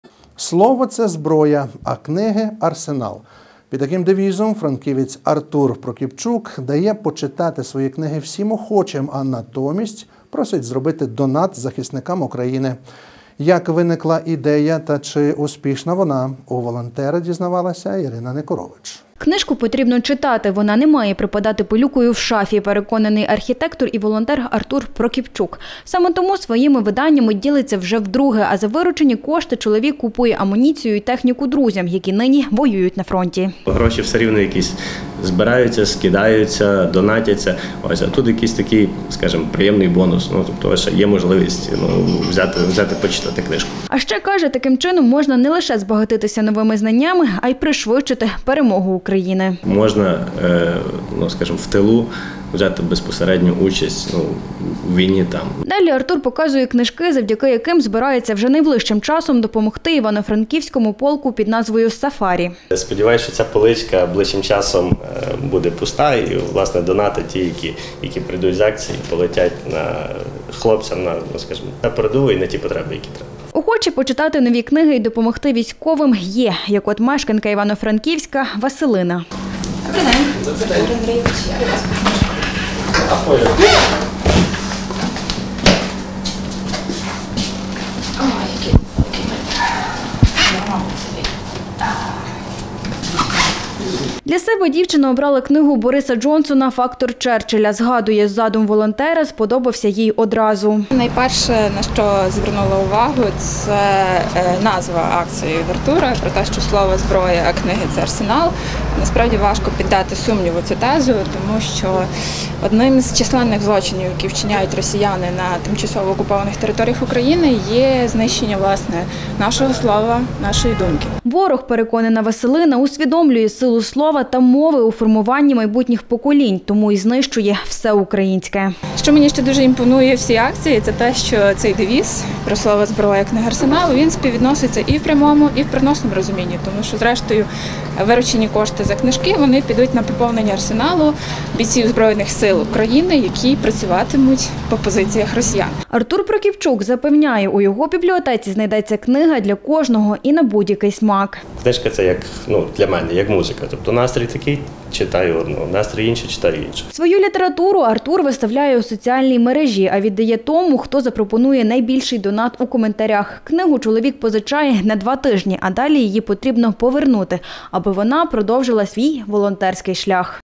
Книга за донат для ЗСУ
Слово — це зброя, а книги — арсенал. Під таким девізом франківець Артур Прокіпчук дає почитати свої книги всім охочим, а натомість просить зробити донат захисникам України. Як виникла ідея та чи успішна вона — у волонтера дізнавалася Ірина Никорович.